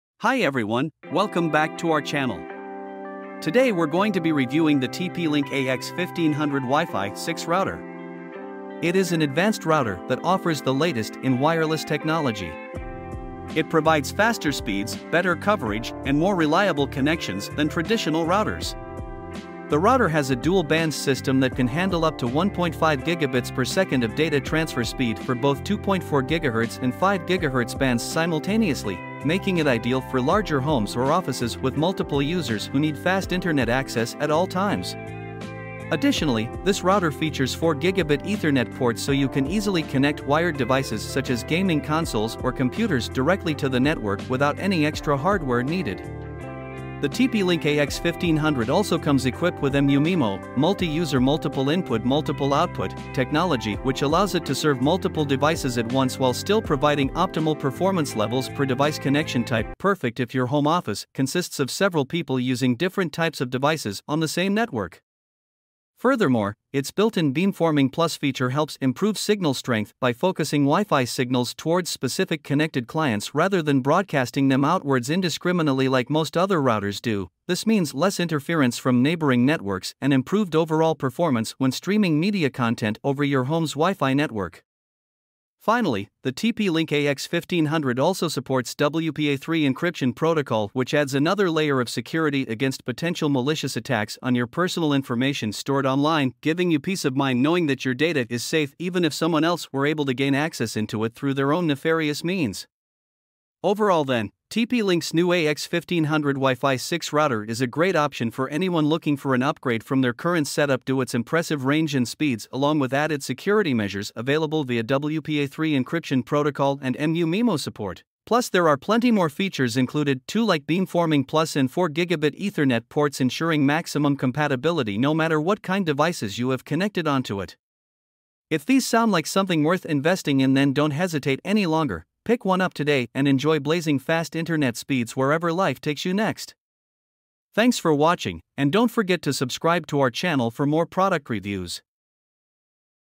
Review the TP-Link AX1500 Wifi 6 Router (Archer AX10)
TP-Link AX1500 Wifi 6 Router (Archer AX10)

Wi-Fi 6 Technology—Archer AX10 comes equipped with latest wireless technology, Wi-Fi 6, for faster speeds, greater capacity and reduced network congestion.
Next-Gen 1.5 Gbps Speeds—Archer AX10 dual-band router reaches fast speeds up to 1.5 Gbps (1201 Mbps on 5 GHz band and 300 Mbps on 2.4 GHz band)
Connect More Devices than Ever Before—Wi-Fi 6 technology simultaneously communicates more data to more devices using OFDMA and MU-MIMO while reducing lag.
Triple-Core Processing—The 1.5 GHz tri-core processor ensures communications between your router and all connected devices are smooth and buffer-free.
More Reliable Coverage—Achieve the strongest, most reliable WiFi coverage with Archer AX10 as it focuses signal strength to your devices using Beamforming technology and 4 antennas.
Increased Battery Life—Target Wake Time technology reduces your devices' power consumption to extend their battery life.4
Easy Setup—Set up your router in minutes with the powerful TP-Link Tether App
Backward Compatible—Archer AX10 supports all previous 802.11 standards and all WiFi devices